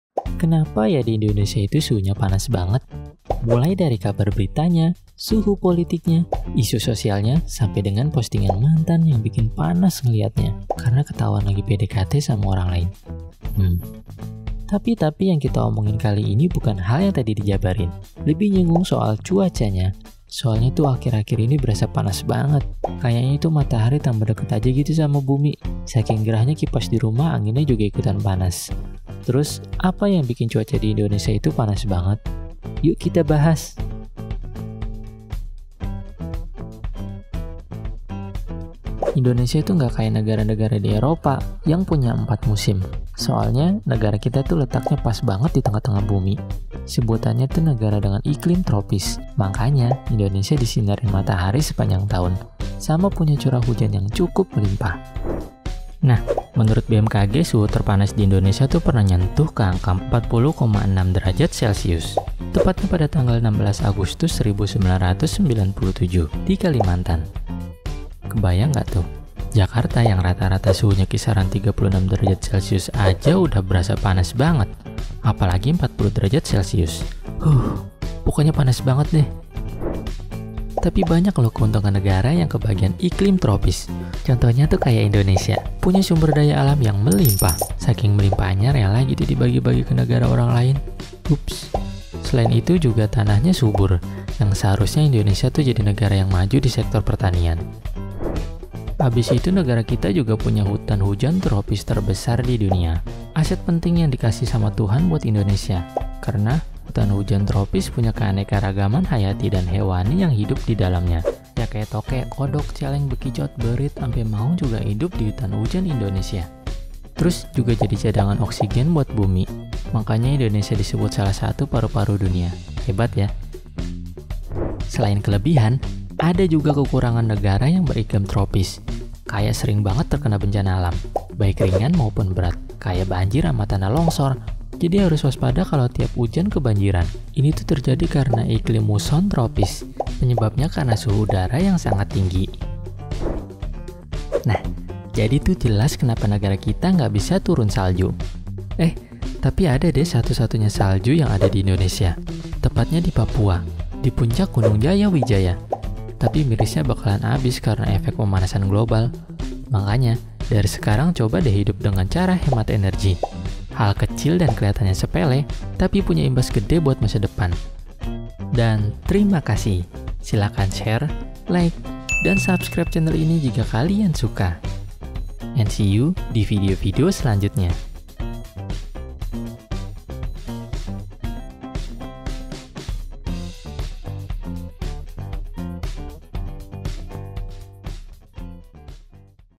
Kenapa sih indonesia beriklim tropis?
Kenapa ya di indonesia itu suhunya panas banget?
Menurut BMKG suhu tepanas di indonesia tuh pernah nyentuh ke angka 40.6 Celsius. Tepatnya pada tanggal 16 agustus tahun 1997 di kalimantan
dan salah satu penyebabnya adalah indonesia yang terletak di garis katulistiwa, di mana kita tau indonesia beriklim tropis
tapi apakah itu penyebabnya?
simak penjelasannya di video ini ya